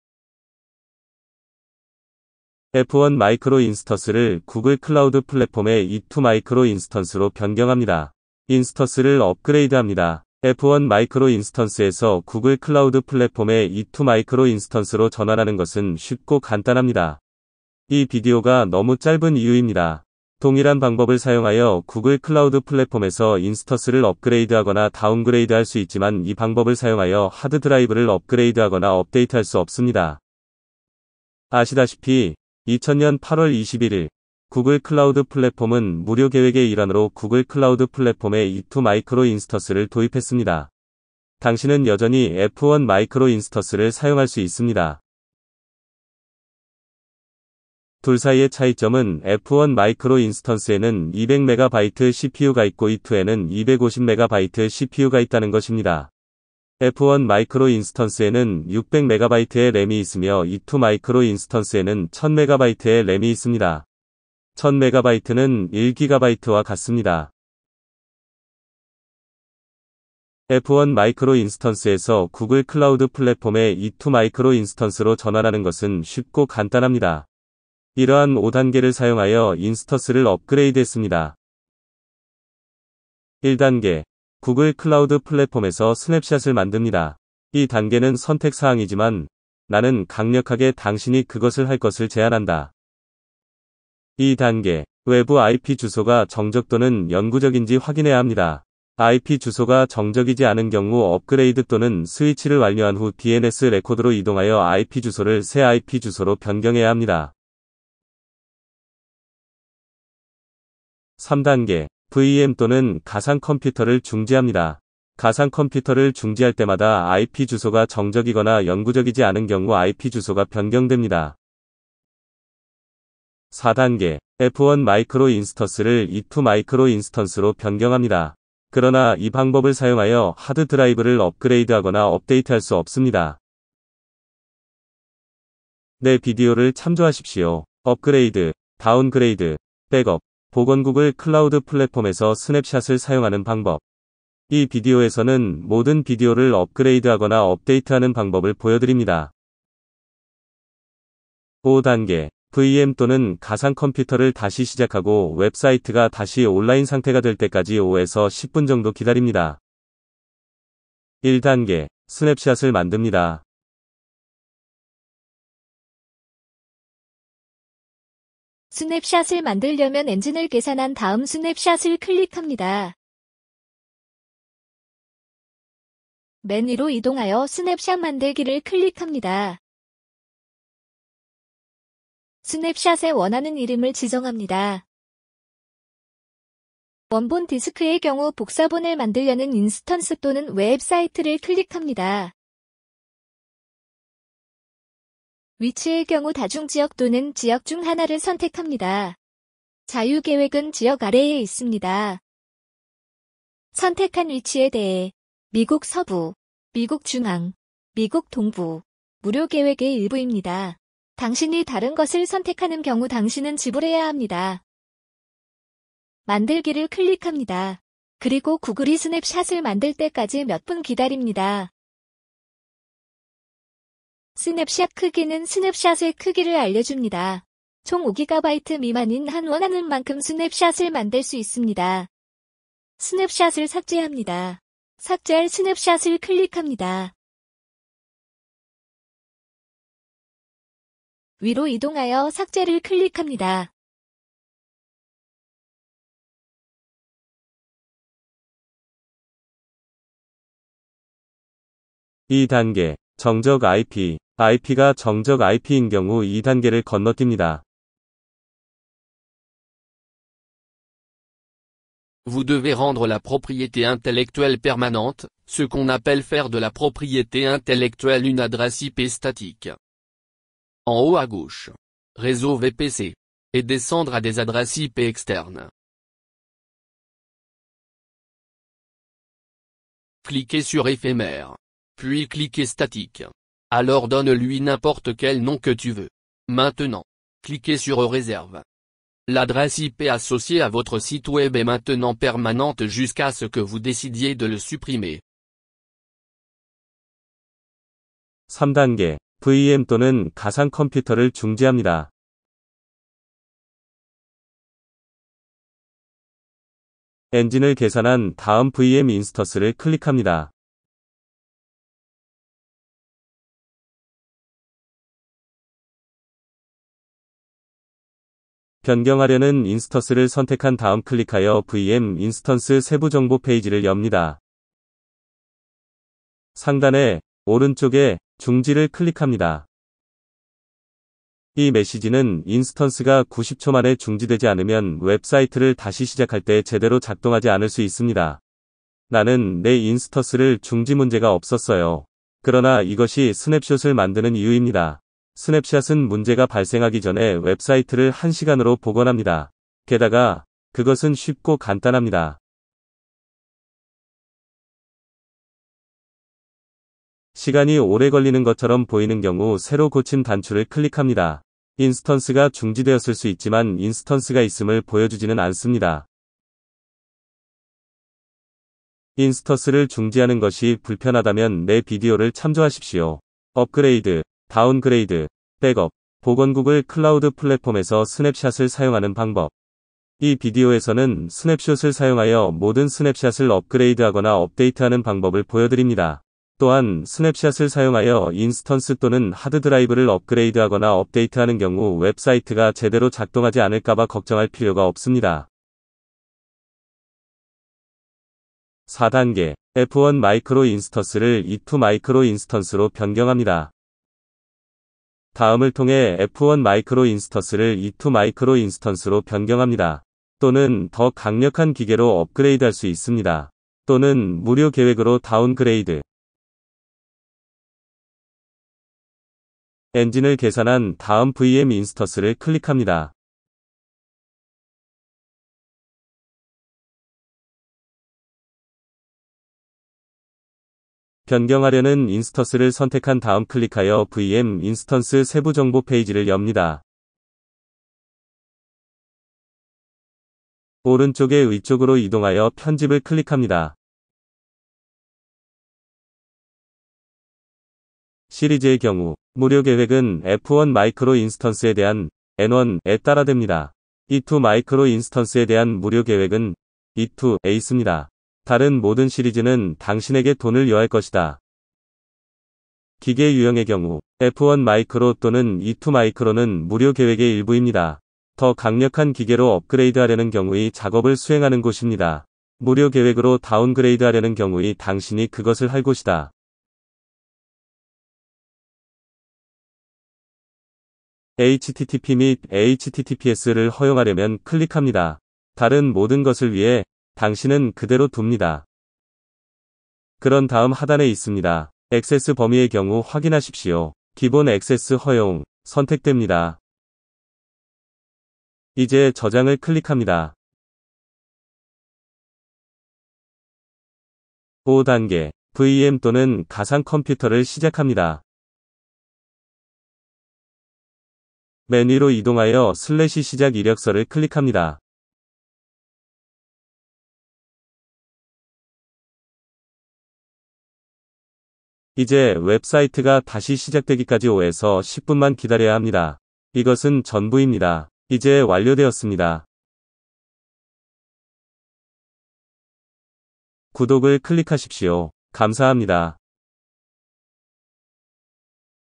Google 클라우드 플랫폼에서 f1 마이크로를 E2 마이크로로 변경합니다. 인스턴스를 업그레이드합니다.
Inrto - 00:00
스냅샷 만들기 – 02:40
정적 IP – 04:11
멈추다 VM = 가상 머신 – 04:57
f1 마이크로 인스턴스를 E2 마이크로 인스턴스로 변경 – 06:37
VM = 가상 머신 시작 – 08:22